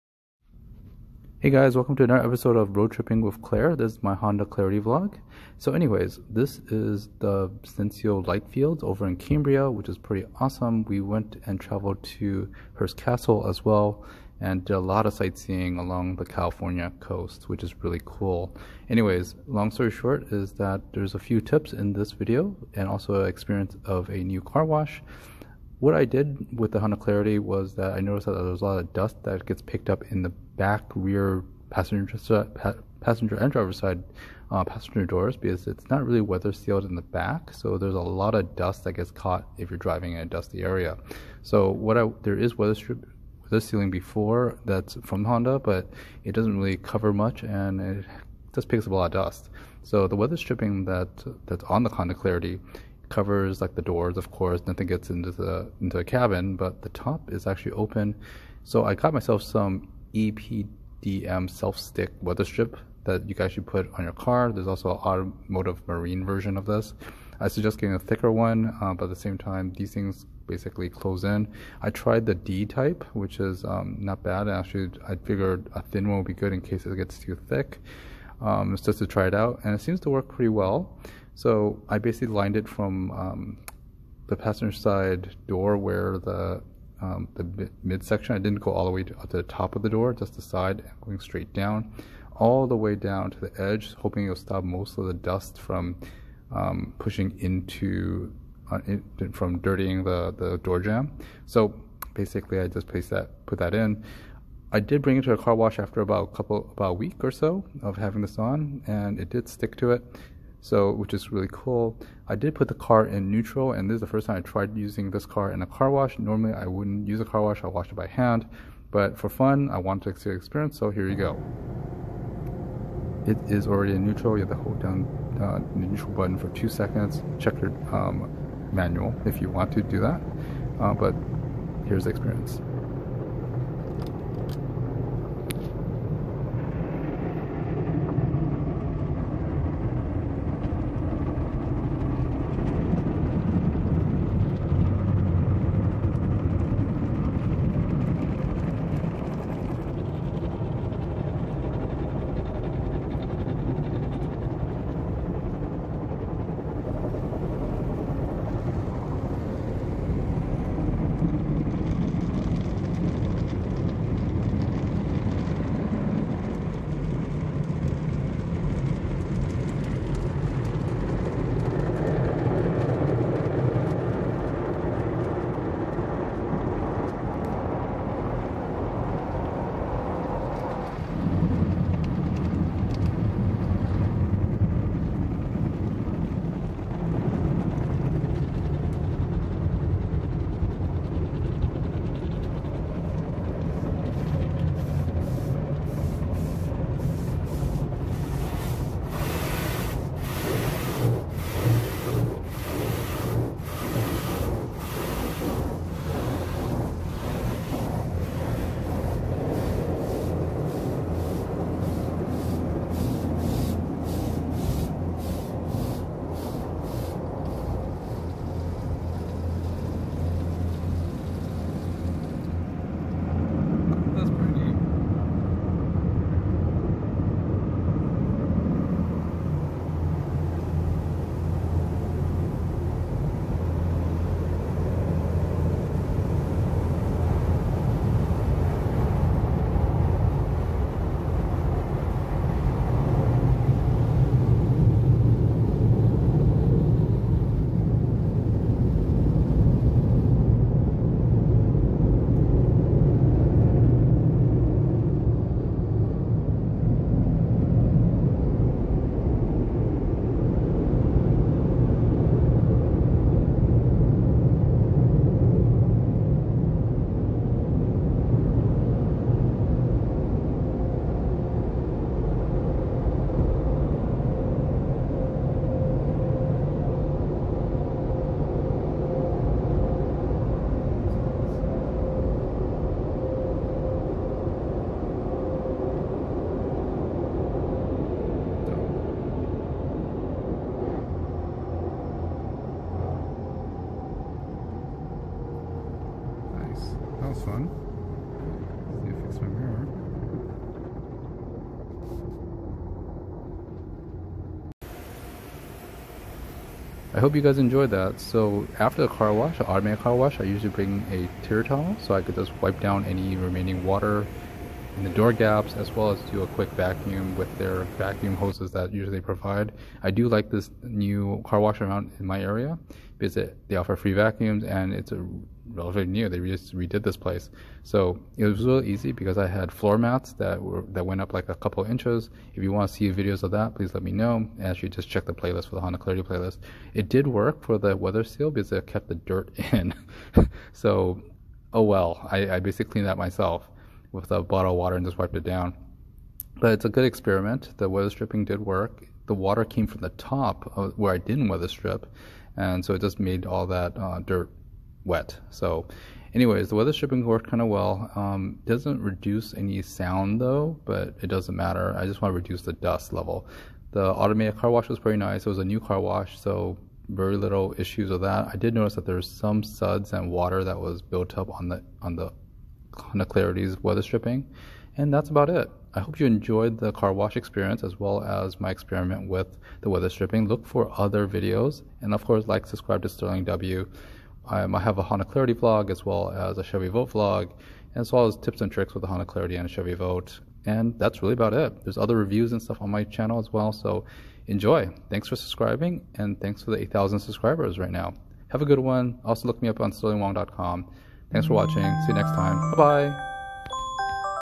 Honda Clarity TIP: Car Weather Stripping & POV Car Wash Experience!
HCV 17 - Honda Clarity VLog 17 - I am adding more weather stripping to lower the dust behind the rear door jams. It seem to work.  I used EPDM "D" type weather stripping ) but there are other types of automotive weather stripping .

After adding the extra weather stripping on the rear doors, I brought my Clarity to the local car wash to test them out.  I worked in keeping the water from getting past the door jam, but since i did not add weather stripping to the top of the door, the jam was still get.  At least I know the experiment worked.

I really love automatic car washes and this new one popped up in Southern California.  I also added of POV video of my car going through  the car wash. Enjoy!